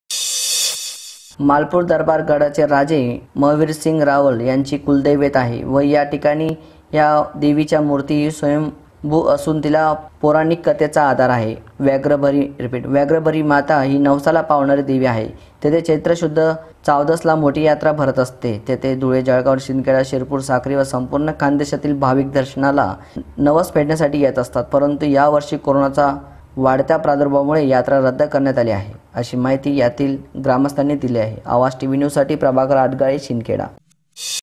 Aawaj Tv
शिंदखेडा | मालपुर येथील हजारो वर्षाची परंपरा दोन वर्षा पासुन  कोरोनाच्या लाॅकडावुन मुळे होते खंडित